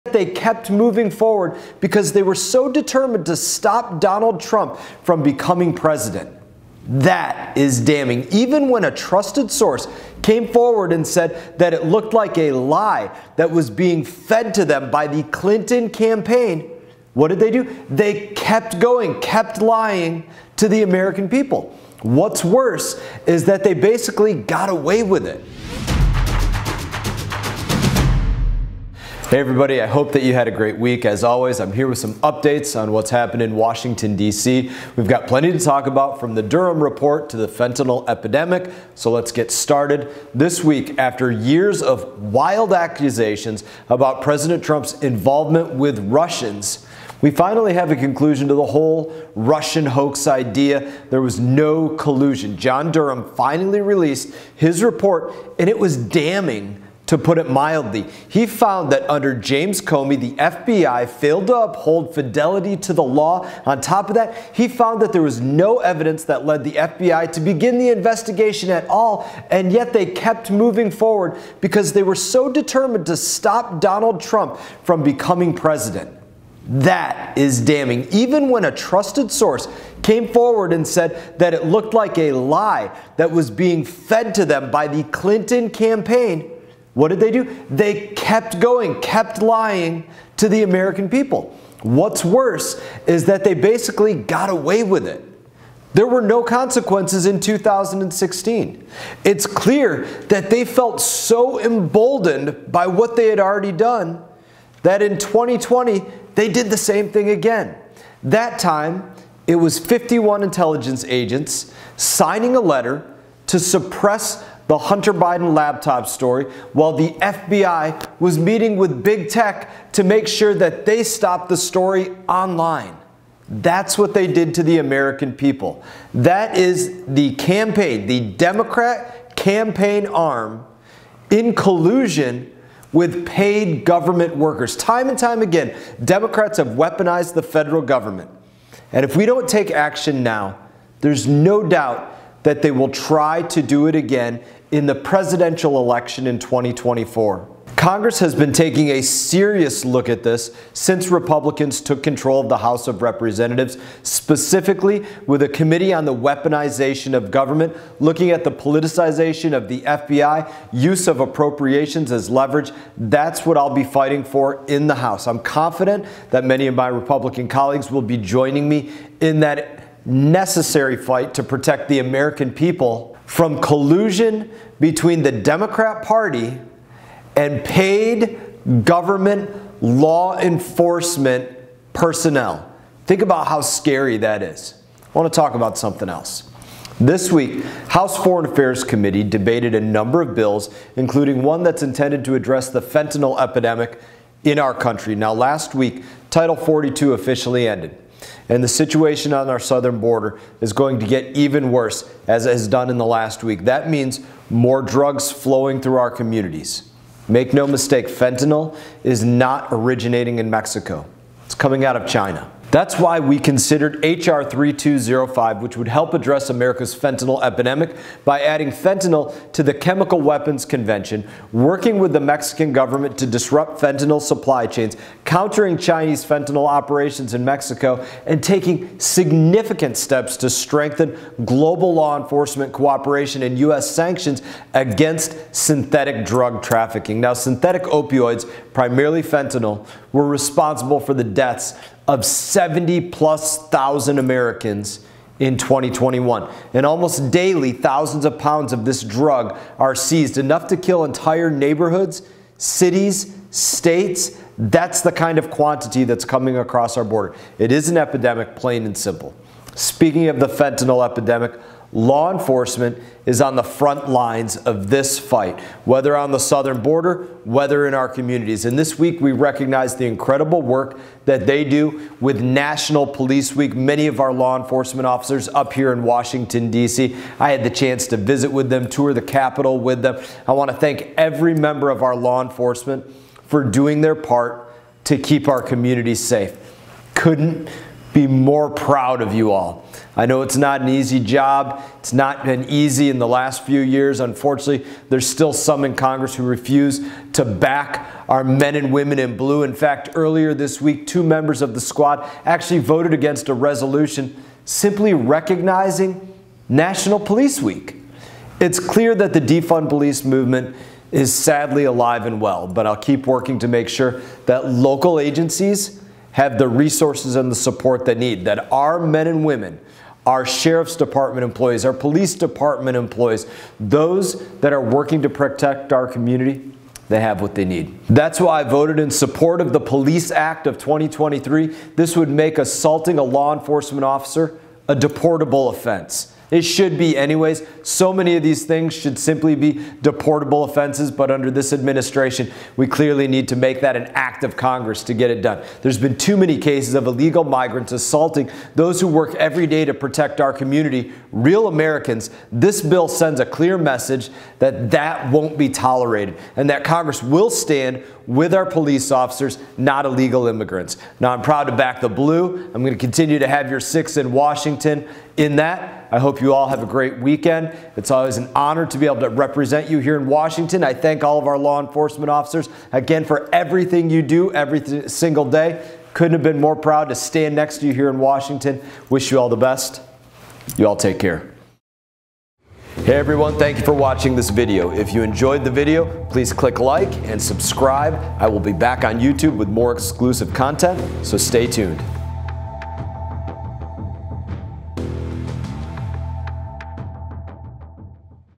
[Week In Review] Standing With The Thin Blue Line | May 12 - 18
This week in Congress, we took steps to ensure local police have the resources they need to do their jobs - from dealing with illegal immigrants to the fentanyl crisis.

Check out the latest WeekInReview for updates, plus my thoughts on the Durham report.